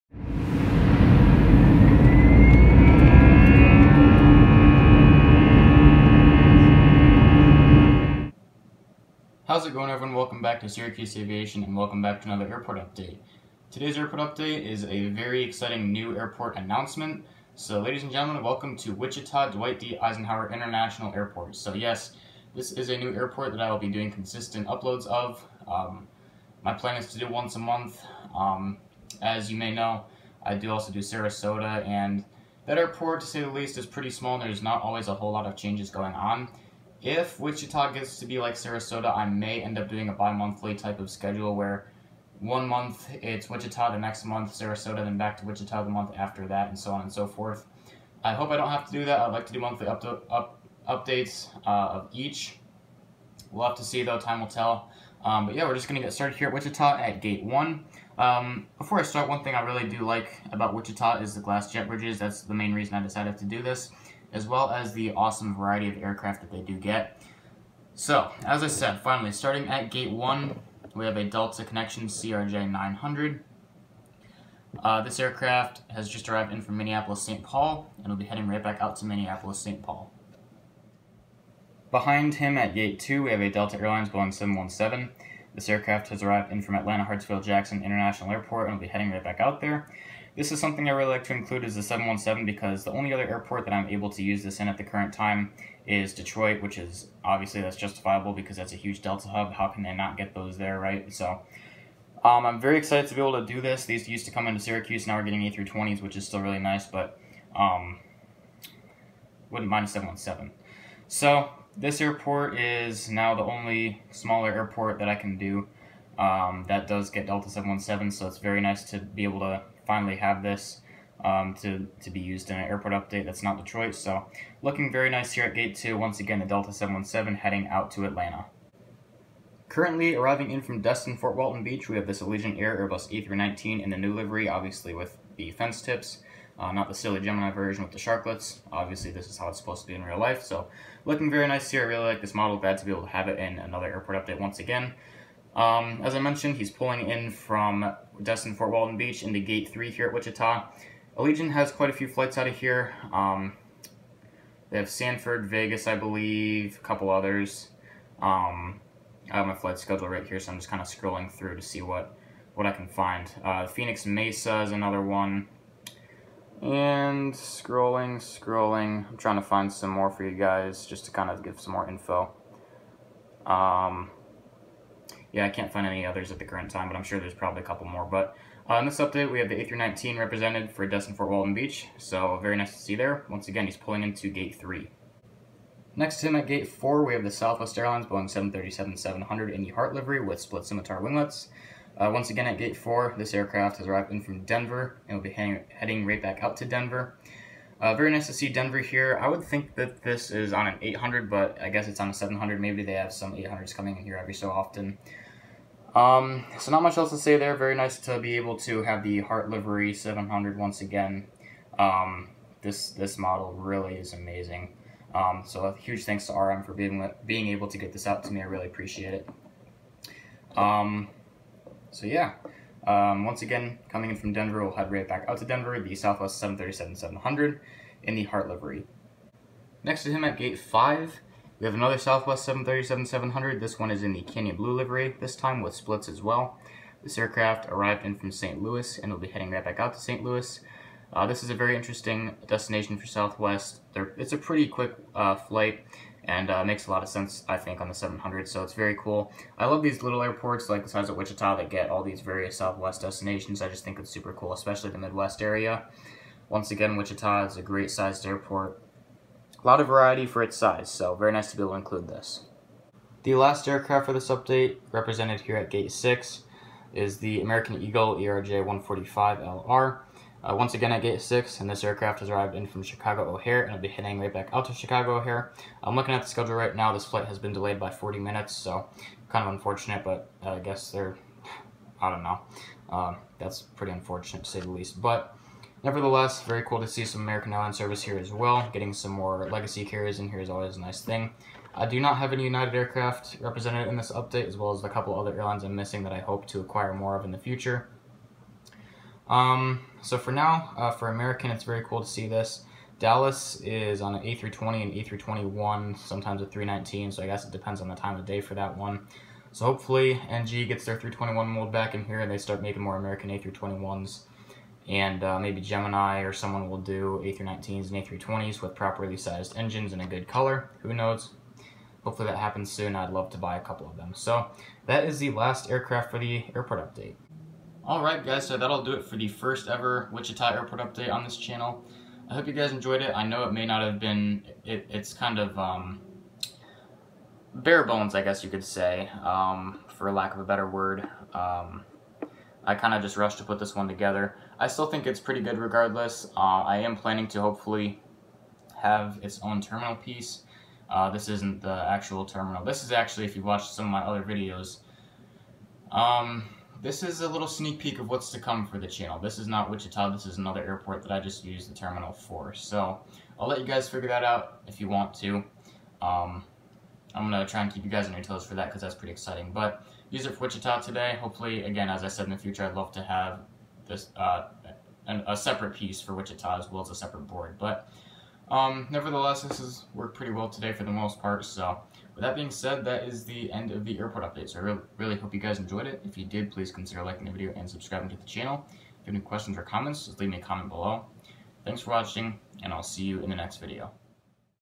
*NEW AIRPORT* Wichita Dwight D. Eisenhower National Airport Update #1 May 2022
Hello, and welcome to a new airport! Today marks the start of a new airport series, Wichita Dwight D. Eisenhower National Airport! I hope you guys enjoy, and I hope you’re all excited for what’s to come!